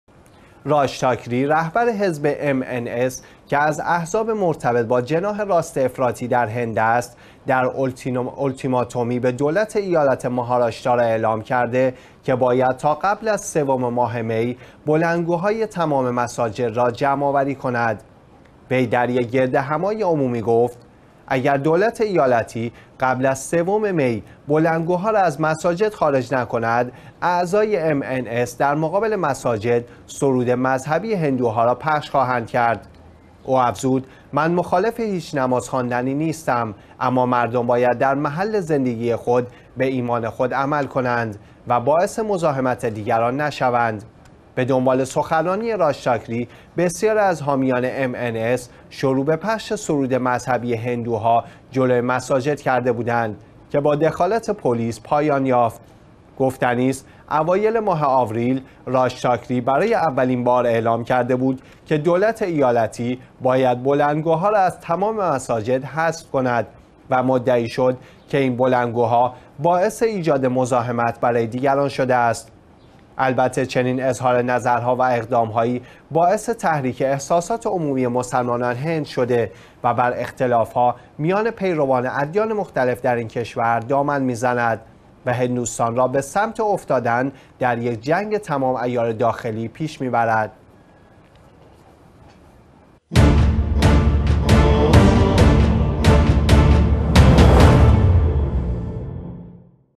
سیاستمدار هندی: بلندگوهای مساجد را جمع کنید
راج تاکری، رهبر حزب MNS که از احزاب مرتبط با جناح راست افراطی در هند است، در اولتیماتومی به دولت ایالت مهاراشترا اعلام کرده که باید تا قبل از سوم ماه می، بلندگوهای تمام مساجد را جمع‌آوری کند.
وی در یک گردهمایی عمومی گفت: اگر دولت ایالتی قبل از سوم می بلندگوها را از مساجد خارج نکند، اعضای MNS در مقابل مساجد، سرود مذهبی هندوها را پخش خواهند کرد.
او افزود: من مخالف هیچ نماز خواندنی نیستم، اما مردم باید در محل زندگی خود به ایمان خود عمل کنند و باعث مزاحمت دیگران نشوند.
به دنبال سخنرانی راج تاکری، بسیاری از حامیان MNS شروع به پخش سرود مذهبی هندوها جلوی مساجد کرده بودند که با دخالت پلیس پایان یافت.
گفتنی است، اوایل ماه آوریل، راج تاکری برای اولین بار اعلام کرده بود که دولت ایالتی باید بلندگوها را از تمام مساجد حذف کند و مدعی شد که این بلندگوها باعث ایجاد مزاحمت برای دیگران شده است.
البته چنین اظهارنظرها و اقدام‌هایی باعث تحریک احساسات عمومی مسلمانان هند شده و بر اختلاف‌ها میان پیروان ادیان مختلف در این کشور دامن می‌زند و هندوستان را به سمت افتادن در یک جنگ تمام عیار داخلی پیش می‌برد.